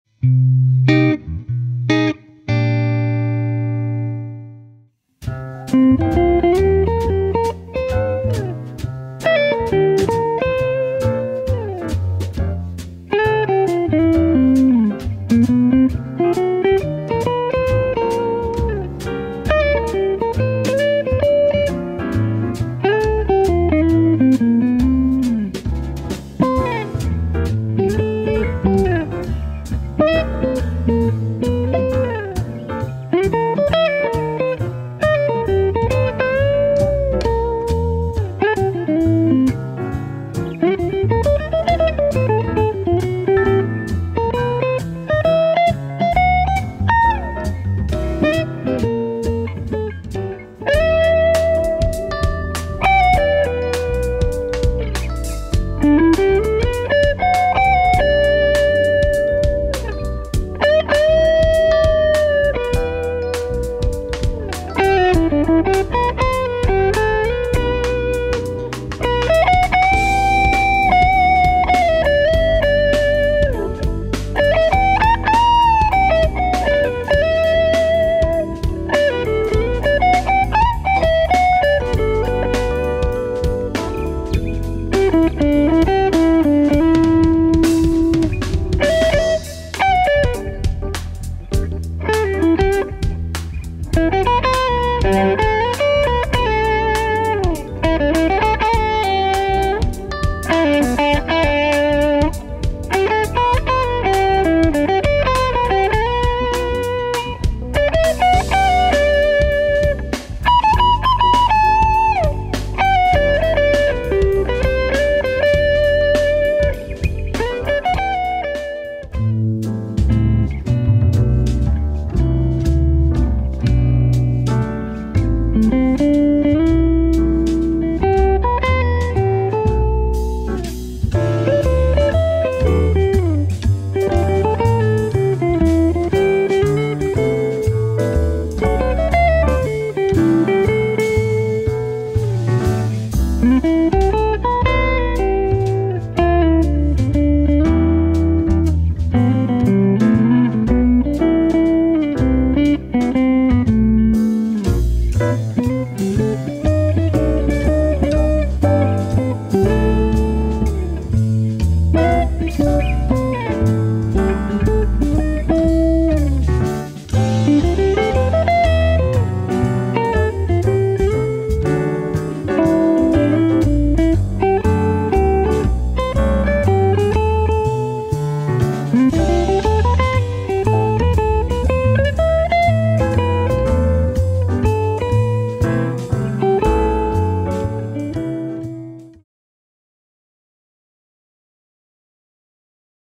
Jaén Guitars - Model: Lisboa & Brunetti Singleman 50
16" Centerblock Thinline Guitar with internal Maple block.
Weight: 3,47kg
Made in Spain
Guitar Player: Jörg Kramer
Amp: Brunetti Singleman 50 Head
Cab: Brunetti 1x12" Singleman Cabinet with WGS 30 Veteran